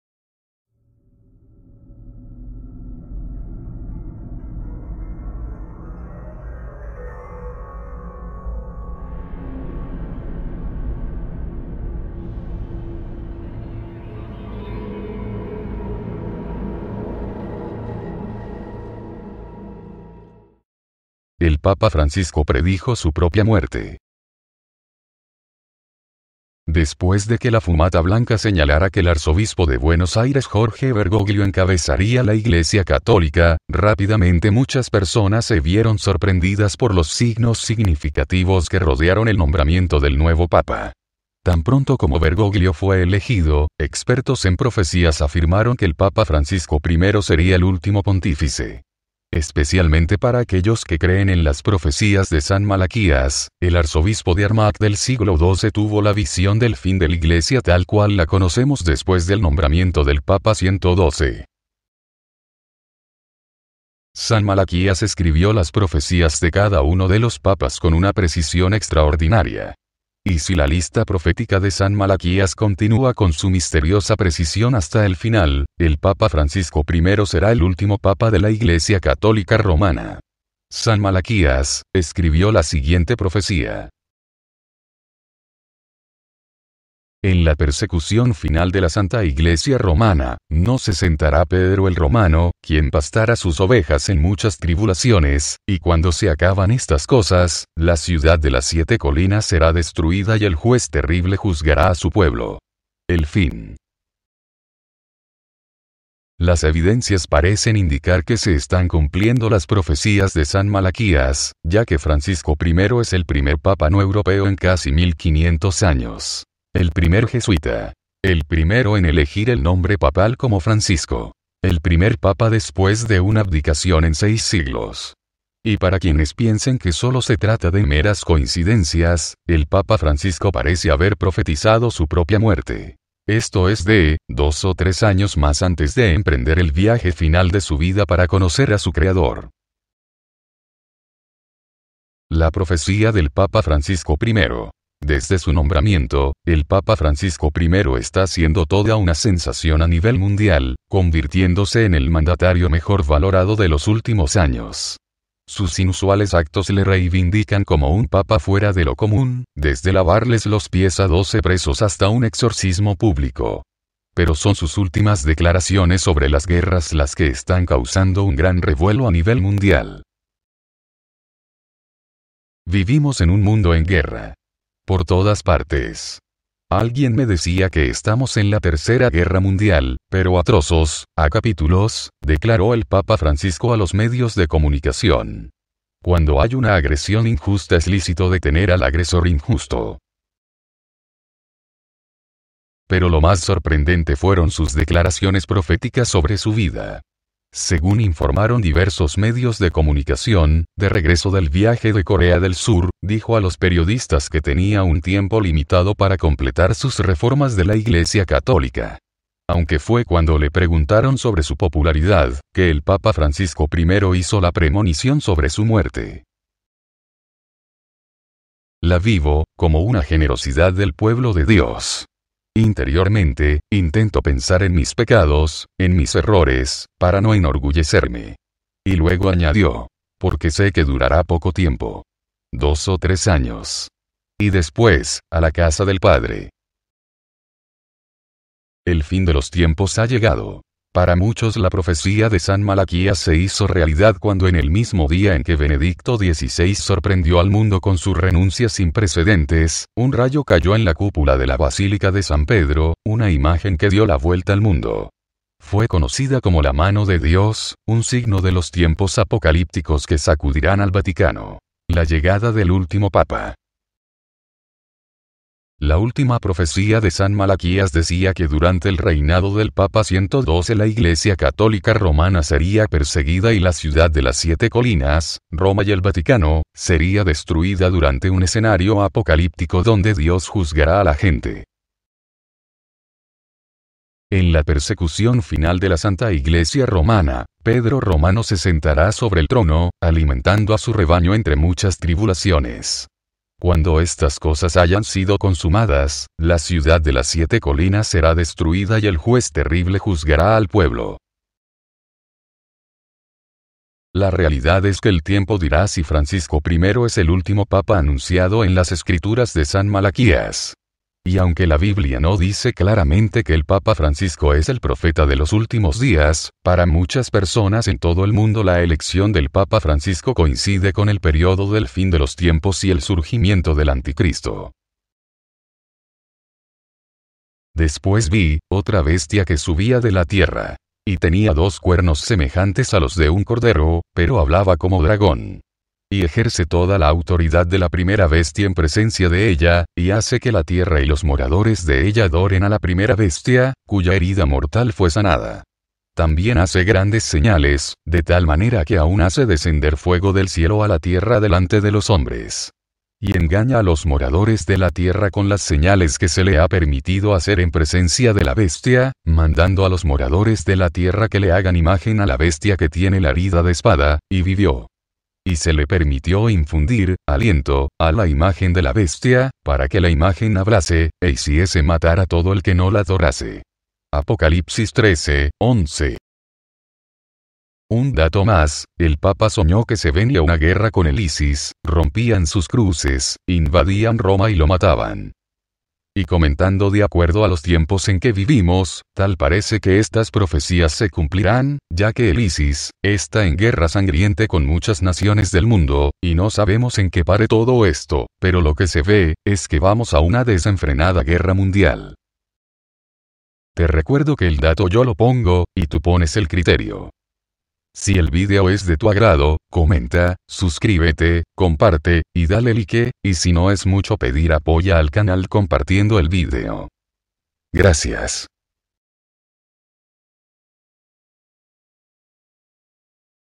Ufo. El Tiempo Llego, El Papa Francisco Profetiza su Muerte, November 2015.
Vivimos en un mundo de guerra Por todas partes, y alguien me comento que estamos viviendo la tercer guerra mundial, pero en trozos, a capítulos.
 Son algunas de las palabras que el papa francisco comento ante varios periodistas,
Un Vídeo interesante que te informara más sobre el momento tan importante de la historia que estamos pasando.

El Dato yo lo pongo y tú pones tu criterio.

Si el vídeo te agrado:

C O M P A R T E
C O M E N T A
Y  D A L E   L I K E
GRACIAS.